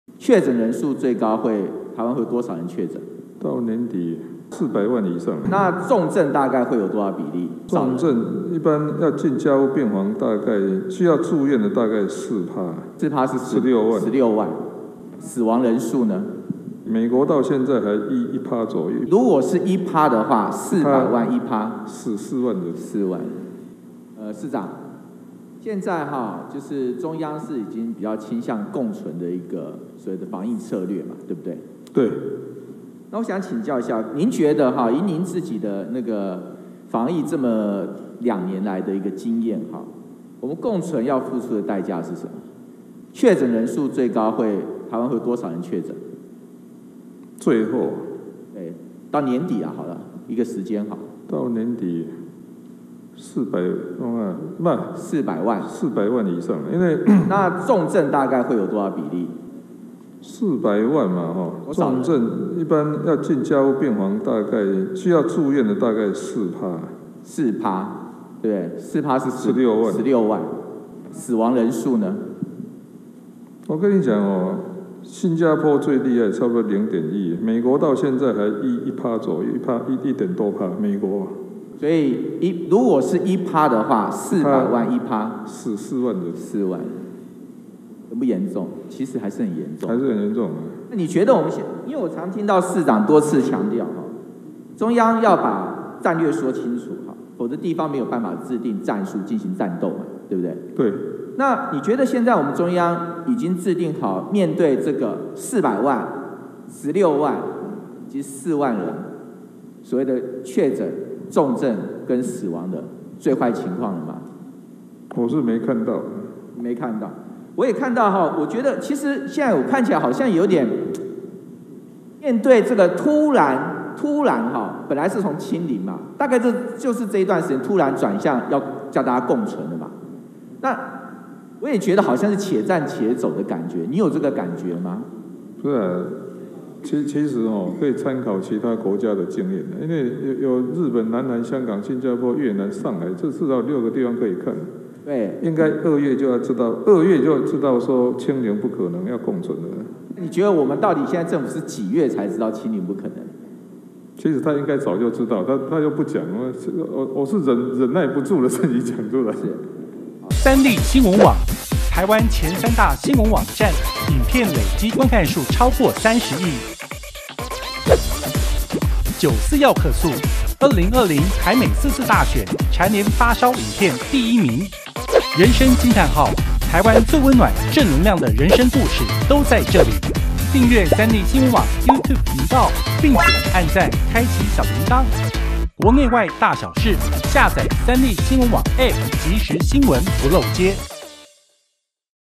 預測疫情共存慘況 柯文哲：年底400萬確診、4萬死亡│政常發揮
友邦連結區
◎ 訂閱【三立新聞網】最新影片搶先看